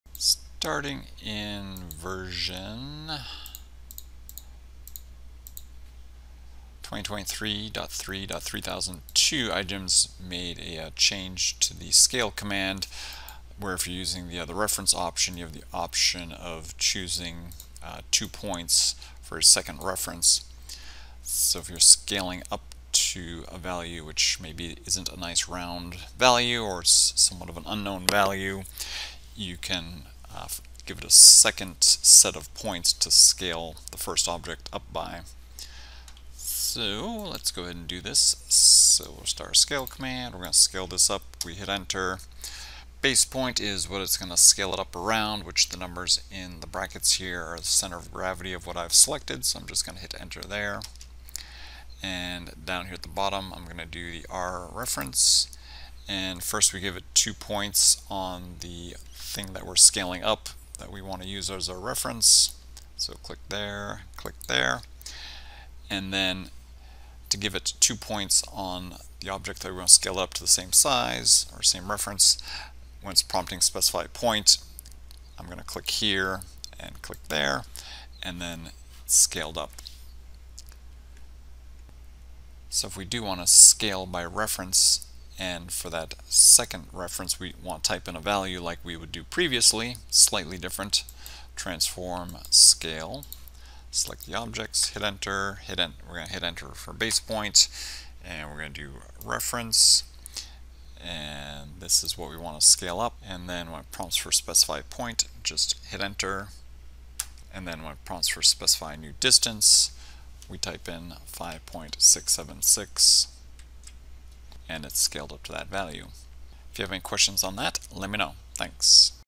IGEMS 2023 Scale by Reference Update
Starting in version R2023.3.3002, IGEMS added the ability to specify two points for a second reference dimension when scaling by reference. This video covers how to do this, and how to type in a number for the second reference.